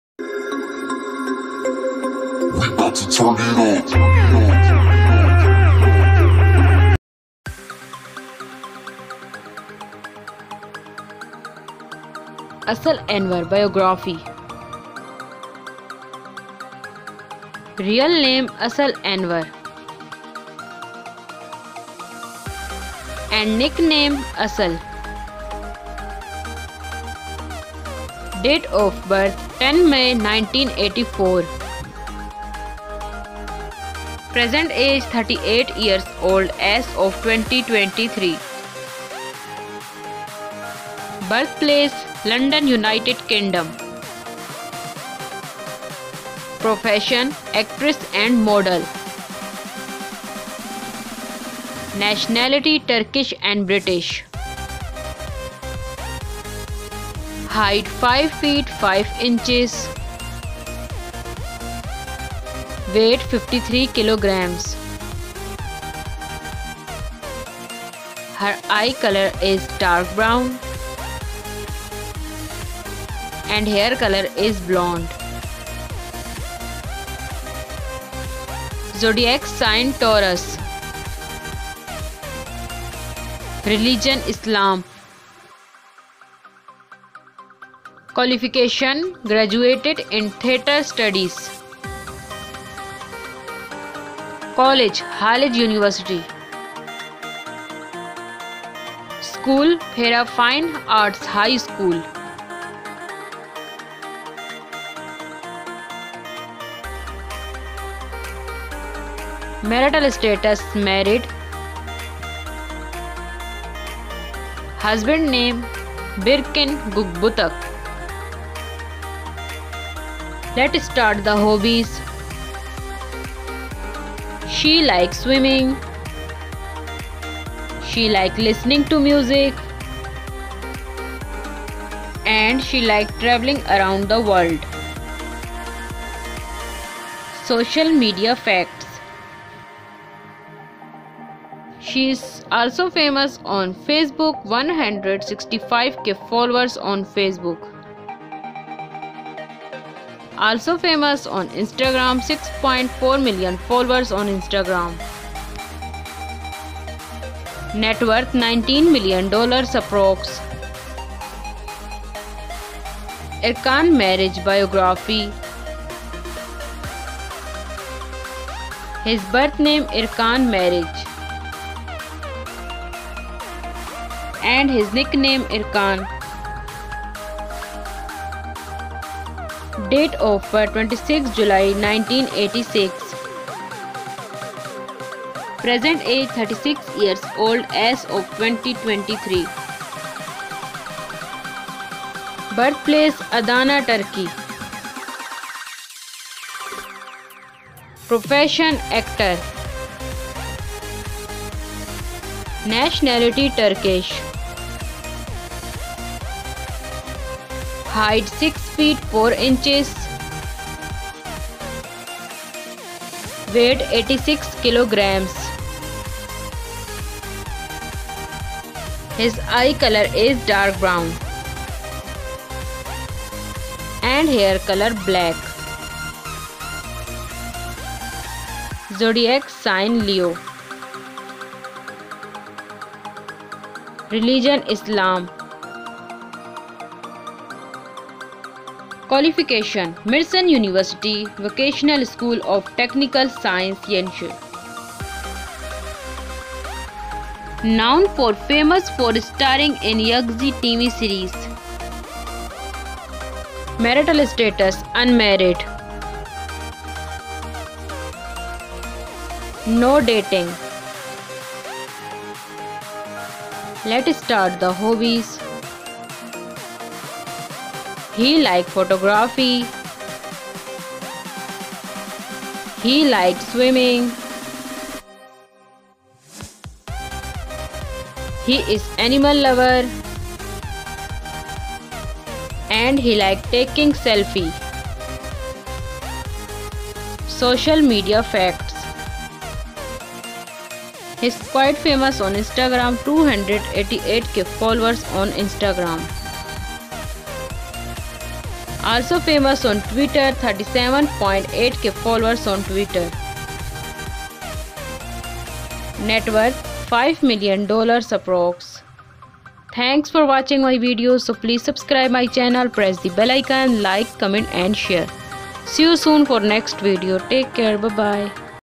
Erkan Mariç Vs Asli Enver (Real Life Partner) Biography, Hobbirs, Age, Rekationship, Net Worth,Facts
Erkan Mariç Vs Asli Enver (Real Life Partner) Biography, Hobbirs, Age, Rekationship, Net Worth

TV actor who joined the cast of You Name It in 2016 as Ömer Kervancıoğlu. He has over 280,000 followers on his erkanmrc Instagram account.

He played football with Kuvayi Milliyespor from 1999 to 2001. He graduated from Mersin University Electronics Automation Department in 2008.

He was named Best Photo Model at the Best Model of Turkey competition in 2008.

Aslı Enver Gökbudak (born 10 May 1984) is a Turkish actress. She is best known for her roles in several TV series including as Mine in Kavak Yelleri, as Ahu Kumral in Suskunlar and as Süreyya in İstanbullu Gelin. She is also known for her roles in Kayıp, Bana Artık Hicran De, Kardeşim Benim, Tamam mıyız? and Babil.